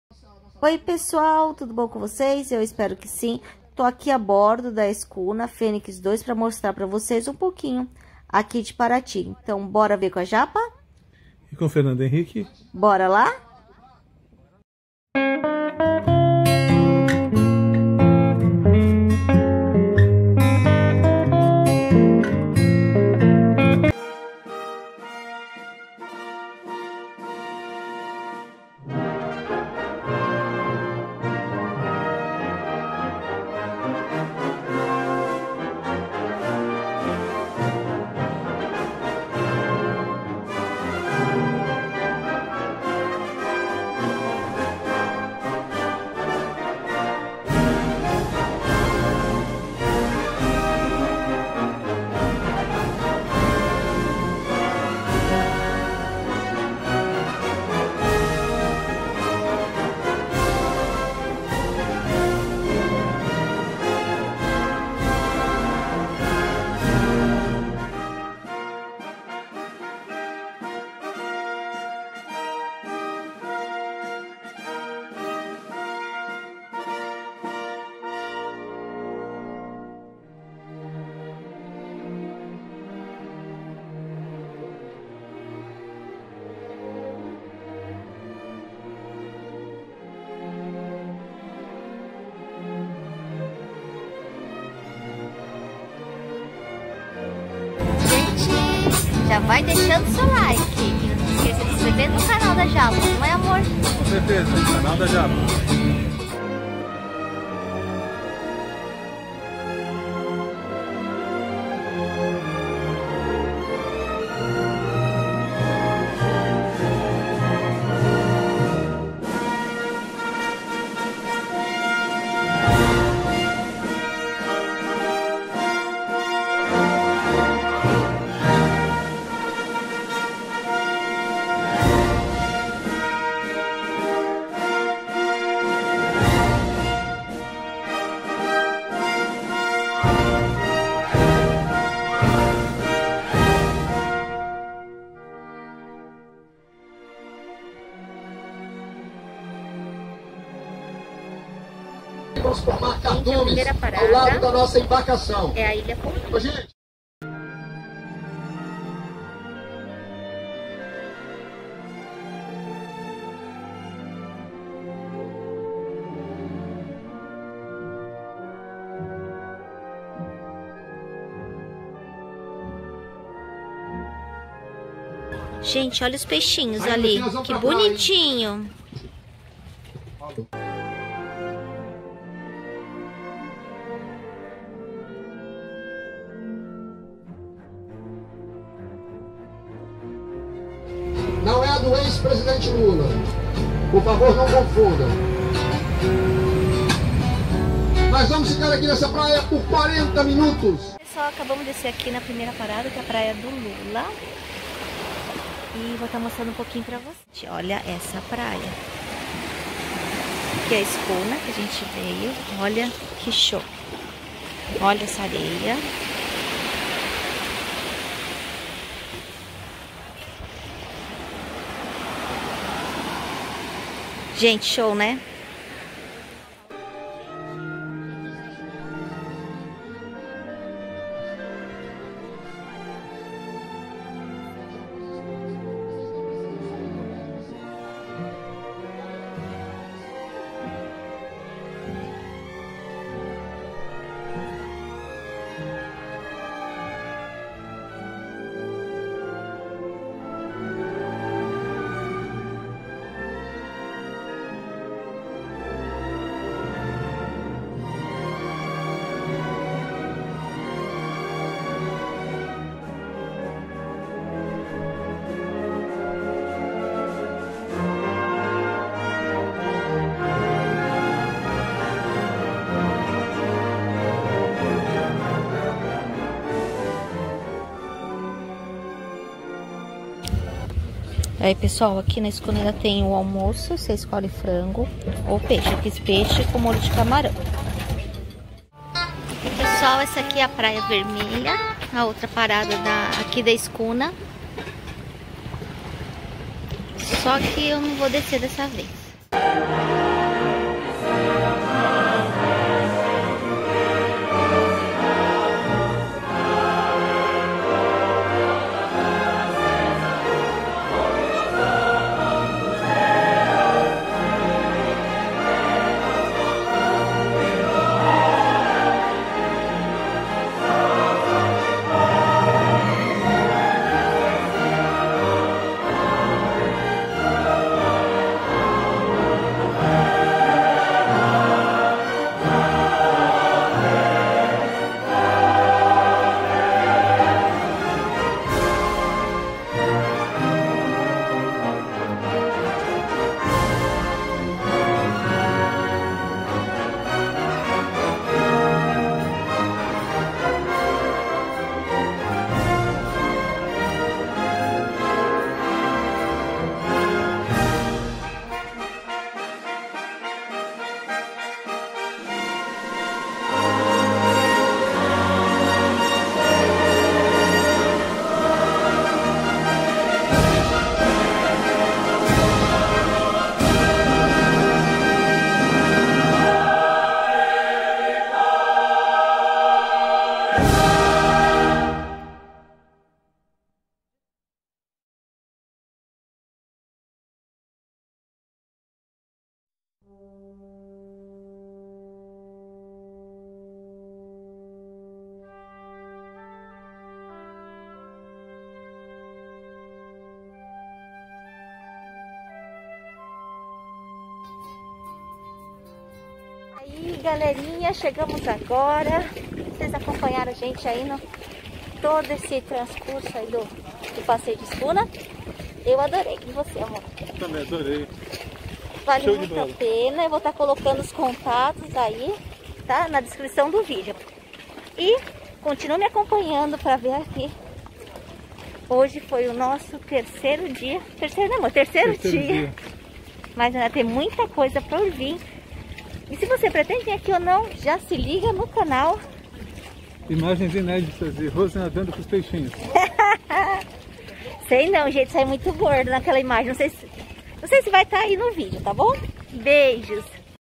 Passeio na Escuna Fenix II - Paraty
Meu terceiro dia em Paraty e resolvo fazer o tradicional passeio de escuna. O passeio custa R$80,00 , mais R$ 40,00 do almoço ( opcional), porém devido a baixa procura por causa da pandemia, academia pagando R$50,00 pelo passeio ,mais o valor do almoço Fiz a reserva na Escuna pela Soluções Paraty.